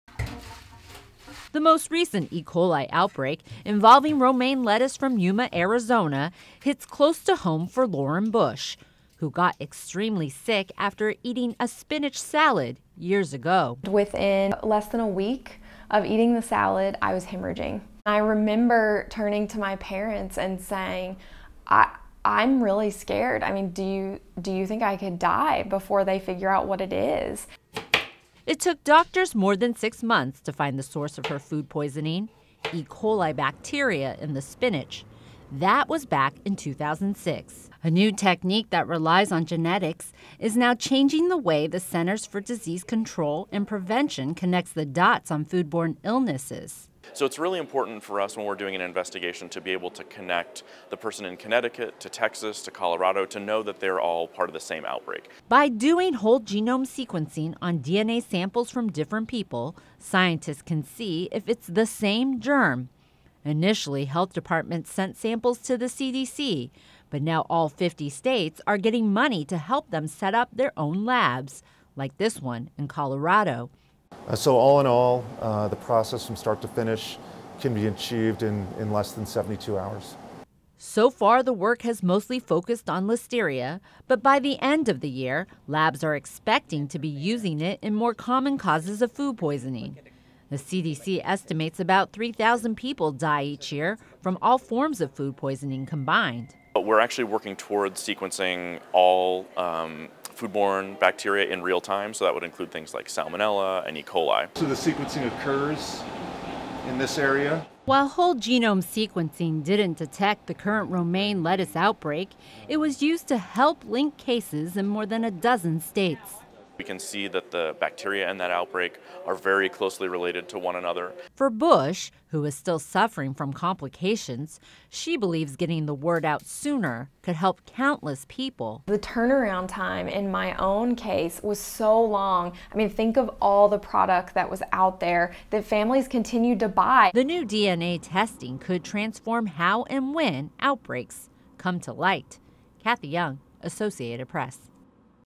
Genetic Sleuthing Helps Food Poisoning Searches
A new technique that relies on genetics is revolutionizing food poisoning investigation and identifying common causes in scattered and seemingly unrelated illnesses. (April 26)